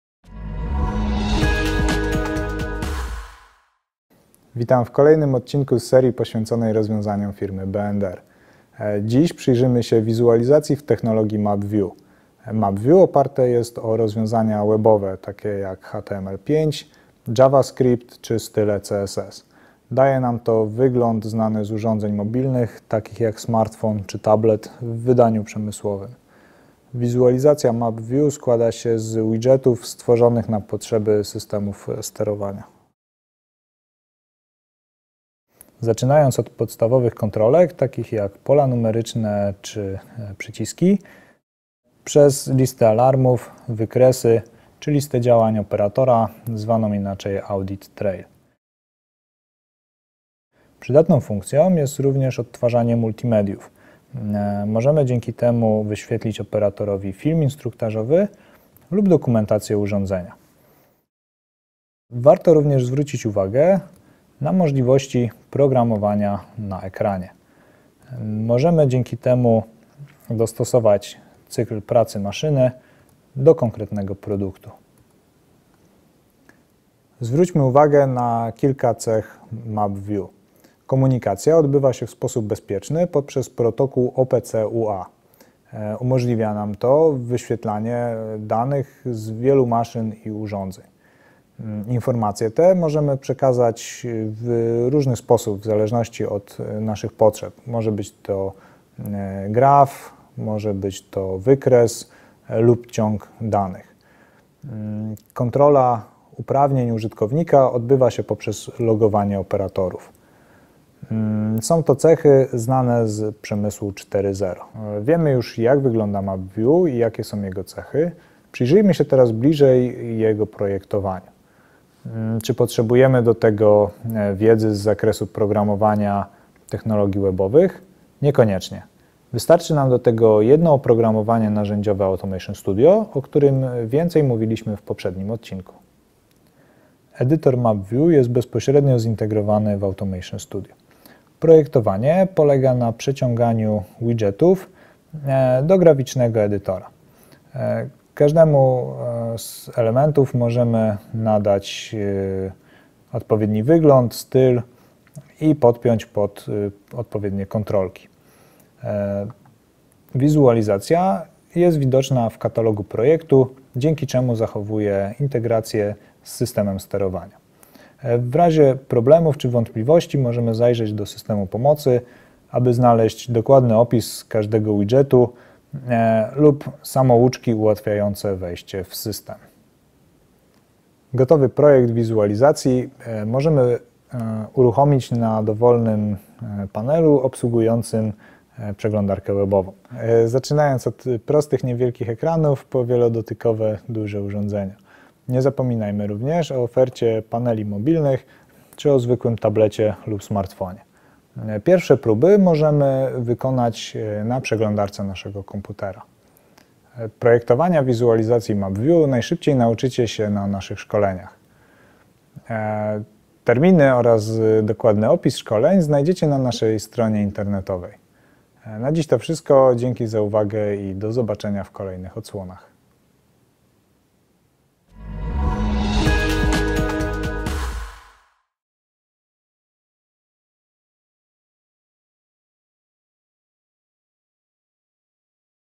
Rozwiązania B&R - realizacja wizualizacji w technologii mapp [audio: PL | sub: EN]
Materiał jest drugim odcinkiem z serii Rozwiązań B&R.
Przybliżamy w nim nowoczesną wizualizację realizowaną w technologii mapp View opartą o komponenty Webowe i stworzoną na potrzeby automatyzacji.
Nowoczesny wygląd i szybkość w realizacji - poznaj mapp View.